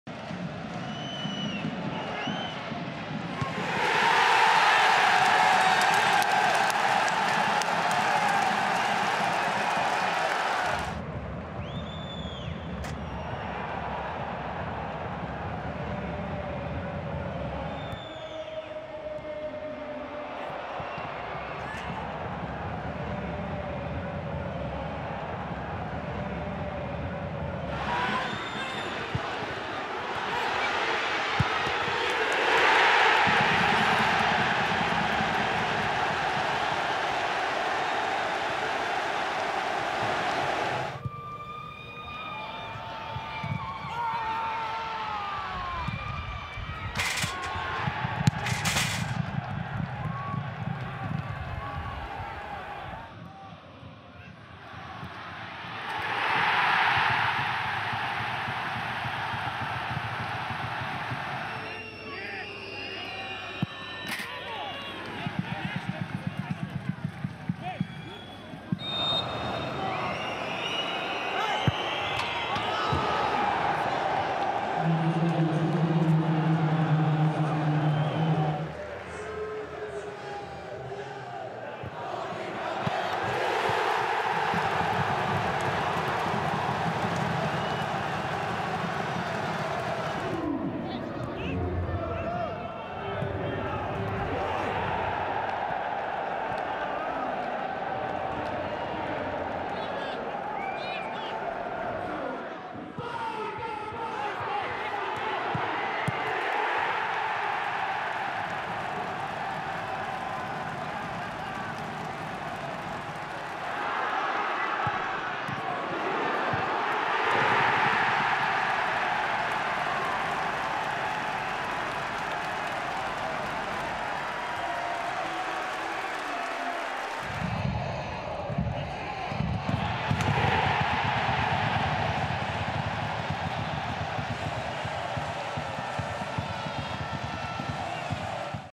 PETTERI FORSELL - 13 GOLI W EKSTRAKLASIE 2018/19
Debiutancki sezon Fina w Ekstraklasie i 13 goli dla Miedzi Legnica! Petteri Forsell zagrał dla beniaminka we wszystkich 37 meczach LOTTO Ekstraklasy 2018/19. Na powitanie strzelił pierwszą i zwycięską bramkę w historycznym debiucie Miedzi w Ekstraklasie przeciwko Pogoni Szczecin, potem dołożył jeszcze 12 trafień i jedną asystę.

Mimo takiego dorobku nie udało mu się utrzymać Miedzianki w lidze na kolejny sezon, o powrót do najwyższej ligi zespół powalczy w Fortuna 1. Liga 2019/20. Chcielibyście ponownie zobaczyć Forsella w Ekstraklasie?

Petteri Forsell - gole w LOTTO Ekstraklasie 2018/19
1. kolejka: Miedź Legnica 1:0 Pogoń Szczecin (90')
6. kolejka: Jagiellonia Białystok 2:3 Miedź Legnica (73', 82')
7. kolejka: Miedź Legnica 2:0 KGHM Zagłębie Lubin (90')
8. kolejka: Wisła Płock 2:2 Miedź Legnica (45')
9. kolejka: Miedź Legnica 1:4 Legia Warszawa (40')
12. kolejka: Zagłębie Sosnowiec 3:1 Miedź Legnica (3')
18. kolejka: Górnik Zabrze 1:3 Miedź Legnica (53')
25. kolejka: Miedź Legnica 3:2 Lech Poznań (36')
26. kolejka: Piast Gliwice 2:1 Miedź Legnica (71')
31. kolejka: Miedź Legnica 2:2 Zagłębie Sosnowiec (4', 45')
33. kolejka: Miedź Legnica 3:2 Wisła Płock (36')

Zobacz więcej!